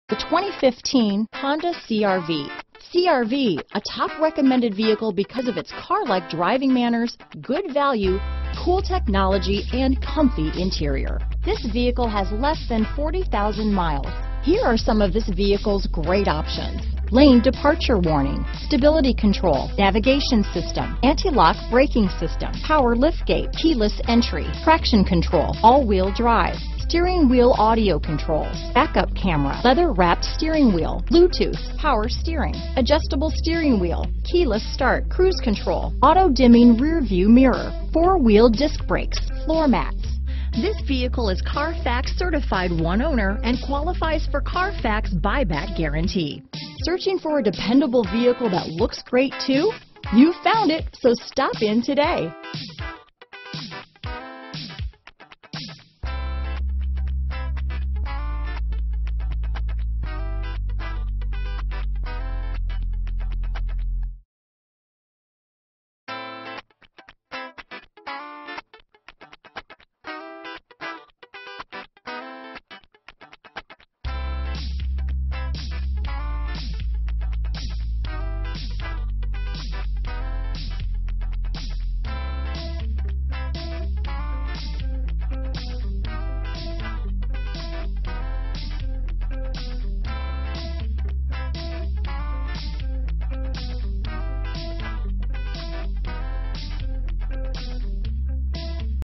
2015 Honda CR-V Glenview, Des Plains, Highland Park, Elmhurst, Morton Grove, IL 51319A
Used 2015 Honda CR-V available in Glenview, Illinois at Castle Honda. Servicing the Des Plains, Highland Park, Elmhurst, Morton Grove, IL area.
2015 Honda CR-V Touring - Stock#: 51319A - VIN#: 5J6RM4H9XFL082262